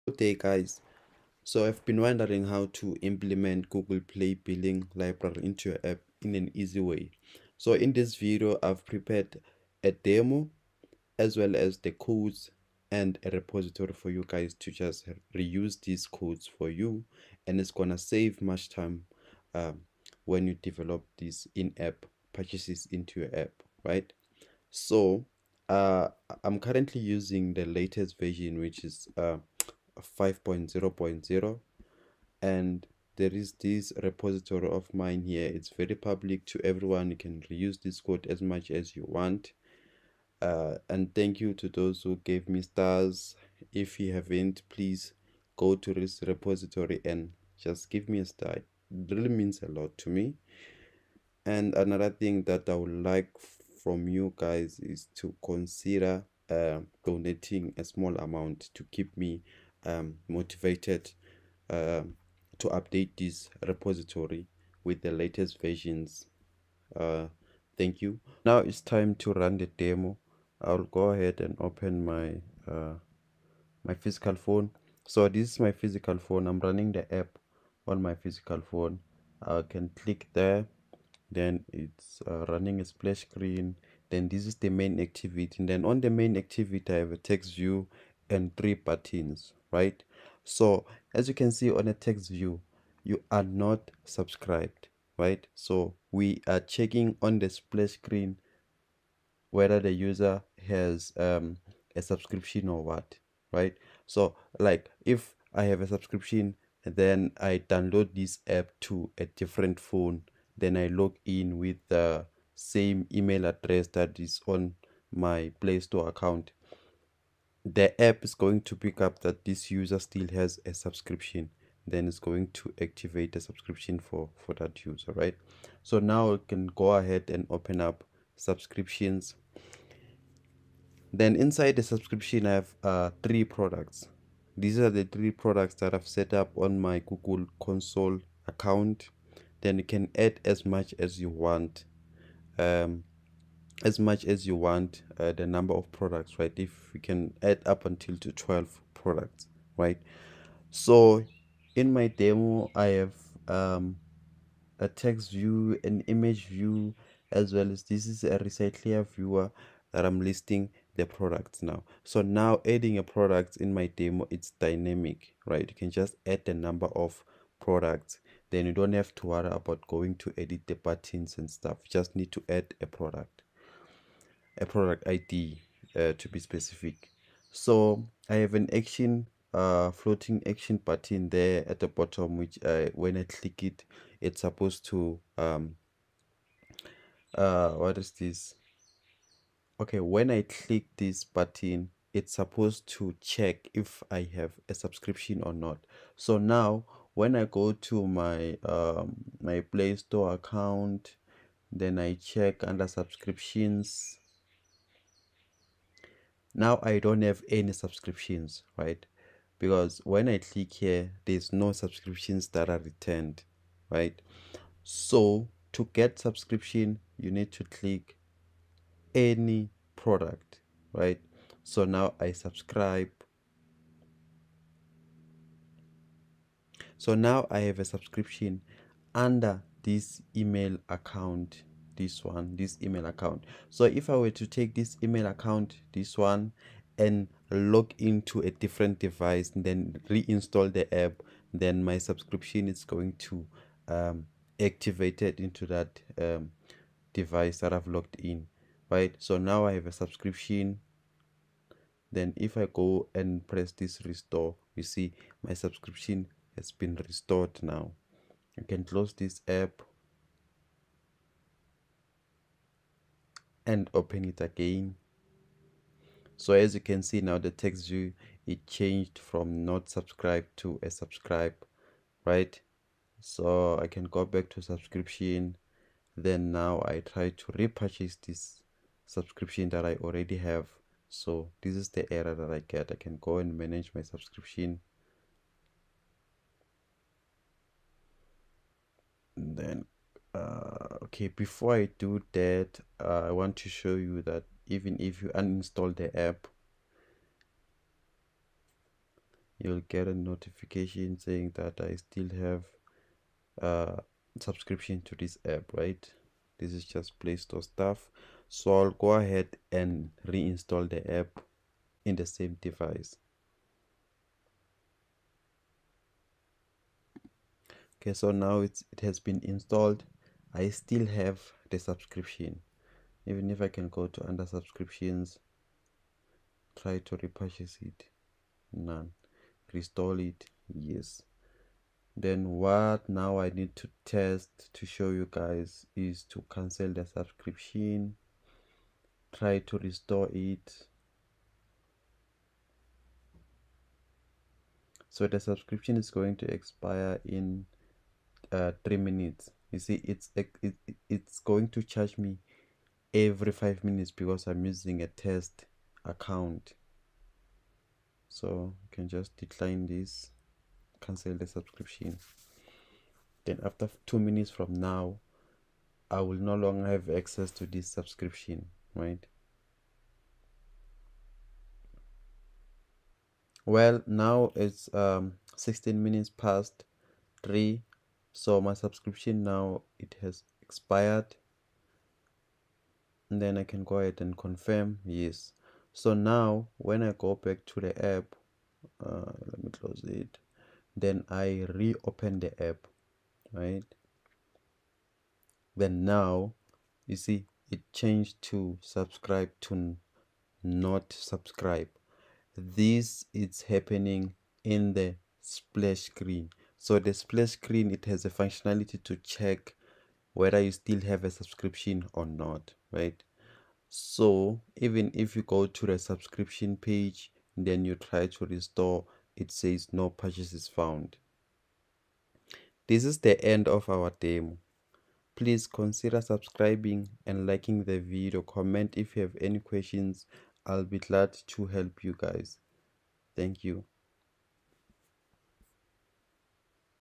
In app purchases Multiple Subscriptions V5.0.0 Demo
Pre-requisite

Google Play Console Account
Published App on Play Store
Tester Device with GMS

Configure Your Testing device by adding the Gmail account to internal testing testers

Setup the in-app purchase subscription product in the Google Play Console account